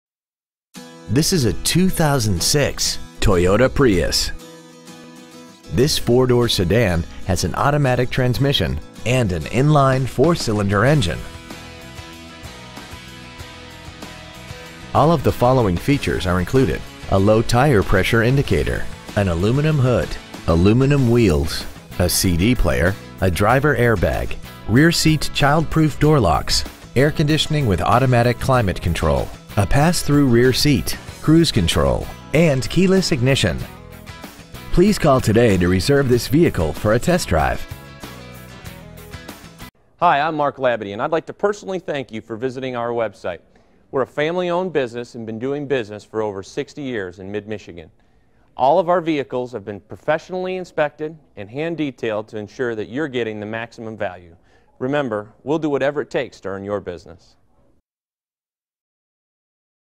Pre-Owned 2006 Toyota Prius Bay City MI
We've been honored to serve the Bay City MI area , we promise that your experience at our dealership will exceed your expectations ! Year : 2006 Make : Toyota Model : Prius Engine : Gas/Electric I4 1.5L/91 Trans . : Variable Exterior : Silver Pine Mica Miles : 140,627 Stock : T010671A Labadie Toyota N Euclid Avenue Bay City , MI 48706 Pre-Owned 2006 Toyota Prius Bay City MI